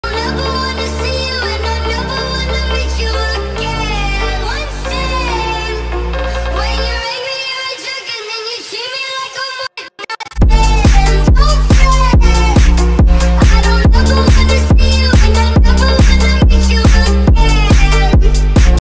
Cet essai du XV de France 🥶
Magnifique essai de France Rugby dans le tournoi des 6 nations

France rugby
Essai d’Antoine Dupont